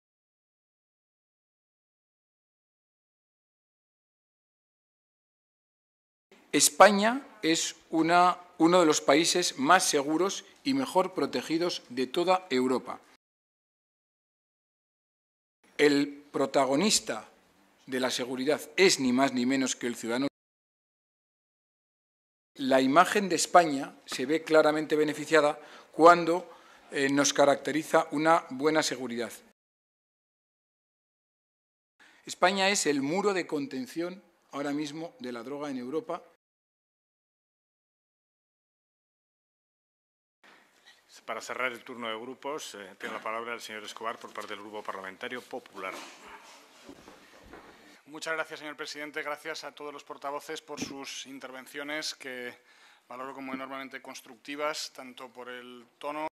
"La imagen de España se ve beneficiada cuando nos caracteriza una buena seguridad"
El portavoz de Interior del Grupo Popular en el Congreso, Conrado Escobar, señala que "España es uno de los países más seguros y mejor protegidos de toda Europa". Así, resaltó que "nuestro índice de criminalidad está 16 puntos por debajo de la media europea, experimentando un descenso en los delitos graves".

También, manifiesta que una España con mayor seguridad mejora nuestra imagen en el exterior, lo que nos beneficia en términos de inversión económica.